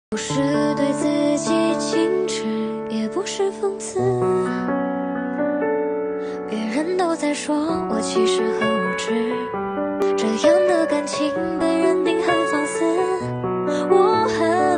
🔥🔥KAIOU Cutting Machine-B4🔥🔥
☄Intelligent infrared induction safety device
☄Intelligent camera position system
☄Intelligent cutting tool combination improve prodution
☄KAIOU design software automatically identify outer contour
☄Metal composite panel better suction effecting
☄Strengthened struture and stable machine body
☄Intelligent feeding, cutting and receiving system
Size:1000*1300mm/1800*1500mm/2500*1600mm/2500*1800mm/3000*2000mm
More channels for info: